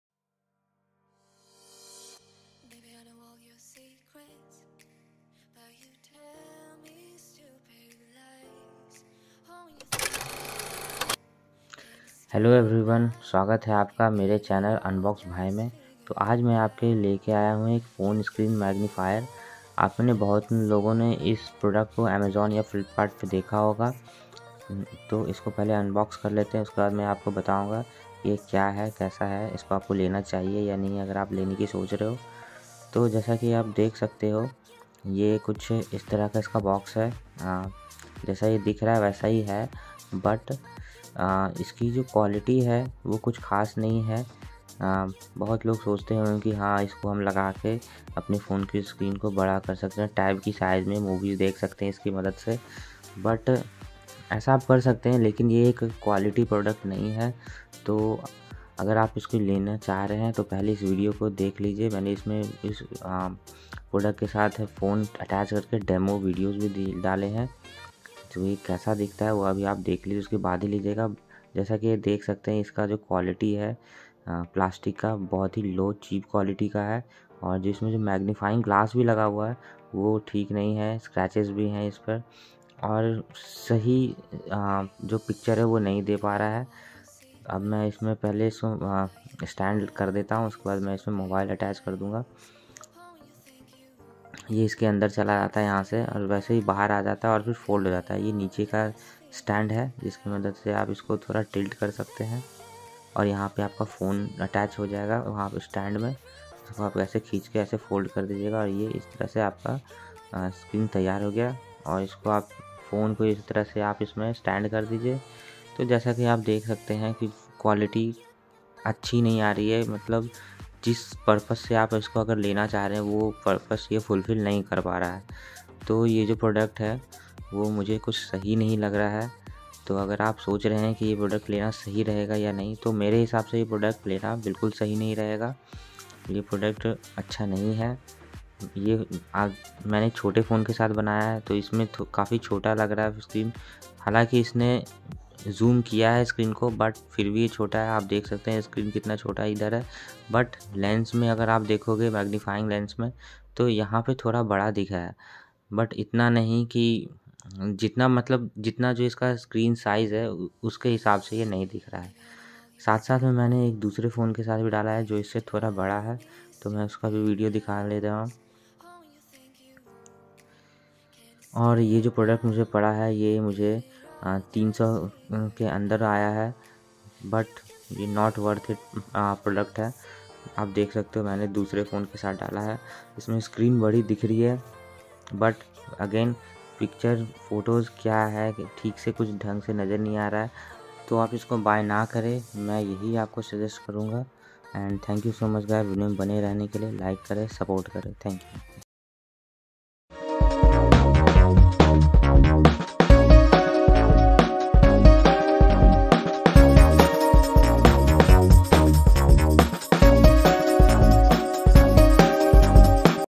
Can You Turn Your Phone Screen Into Tablet ? 🧐
Hello viewers

This video is related to mobile screen magnifier with this product you can watch videos in bigger screen. but this product is comes in cheap quality so i will not recommend you to buy. The purpose of this video is to warn you this is not worth to buy products.